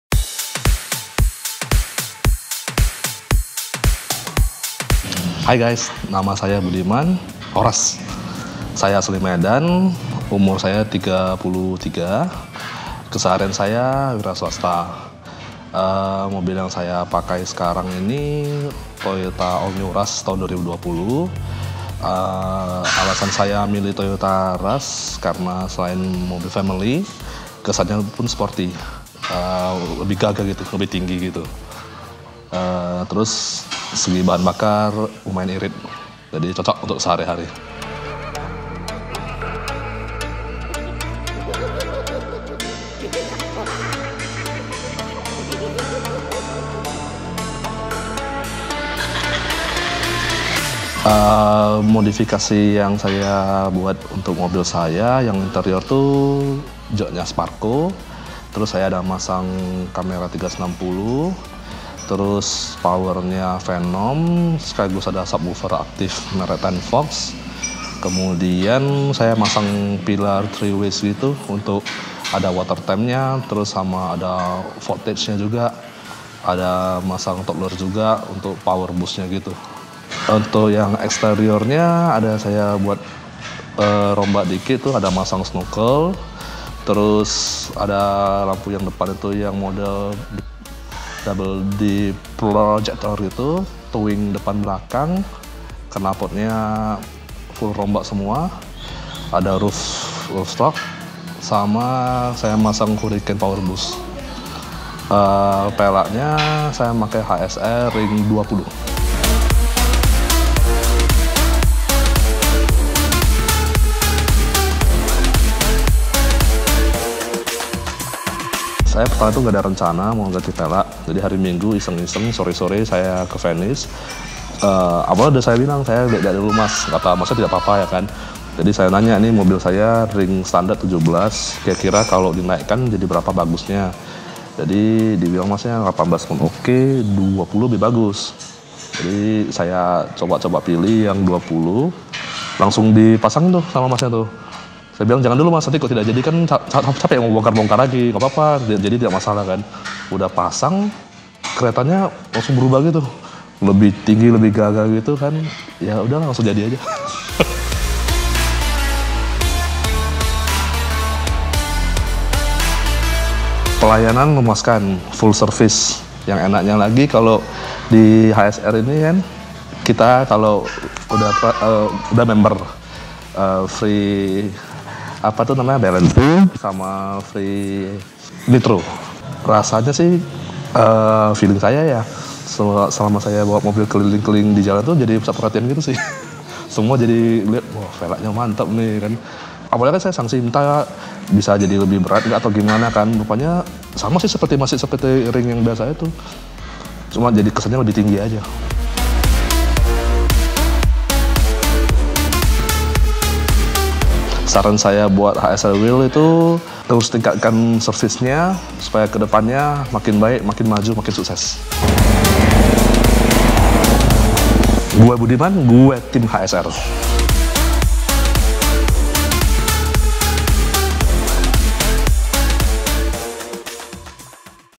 Owner's Spotlight - Modifikasi Toyota Rush Pakai Velg HSR Wheel - Buat Harian
Di Owner’s Spotlight kali ini, teamhsr mendatangi Haiser Medan yang sudah modifikasi Toyota Rush miliknya menggunakan velg
Tertarik dengan velg di video ini? Klik link di bawah.